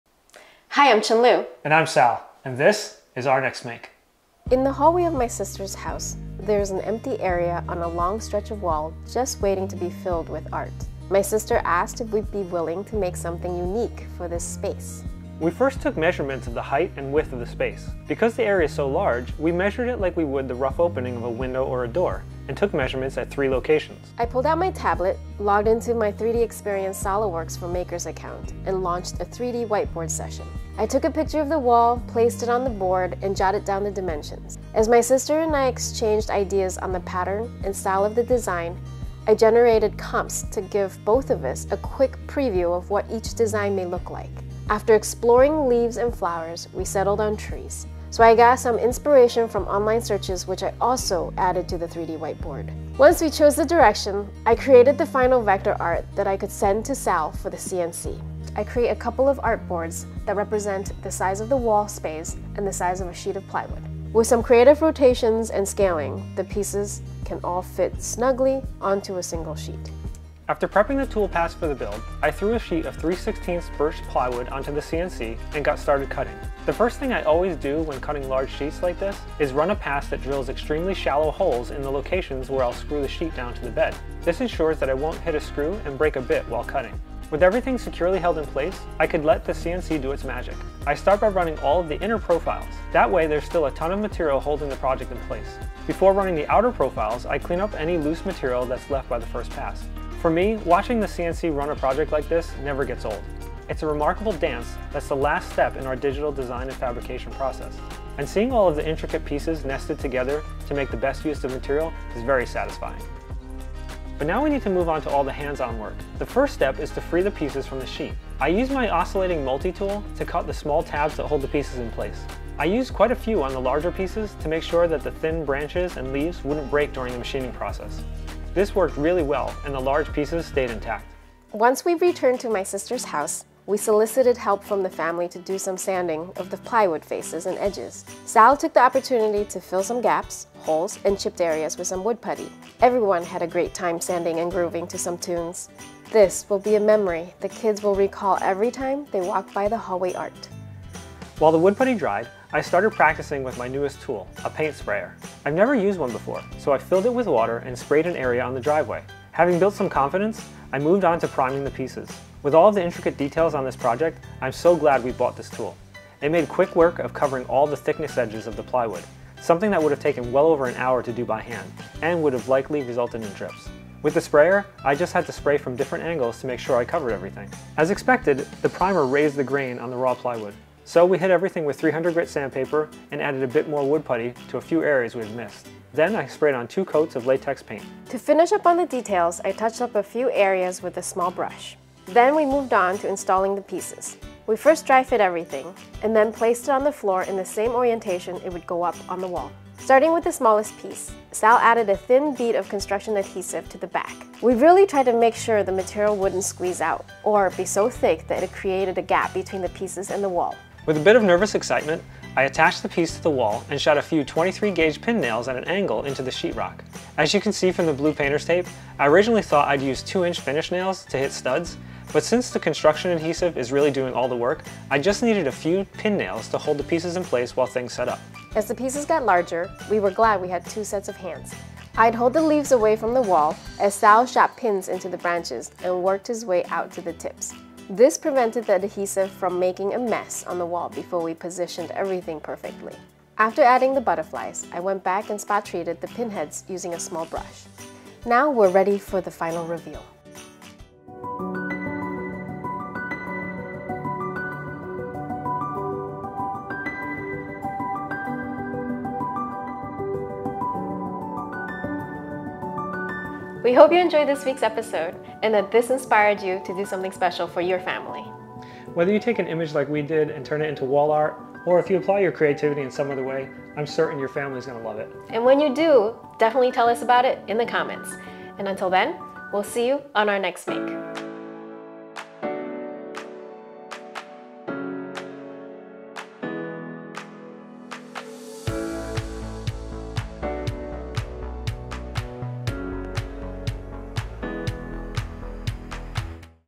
Transforming simple plywood into a beautiful piece of art.
Few things in life are more rewarding than making things for the people you love. This week, we designed a custom piece of wall art and then built it by cutting plywood on a CNC router and finishing it by hand.

*We are SOLIDWORKS employees who love to make cool stuff with the software we help to design.